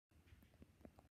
I’m quitting YouTube
I’m done with YouTube I’ll still heart ur comments but no more videos.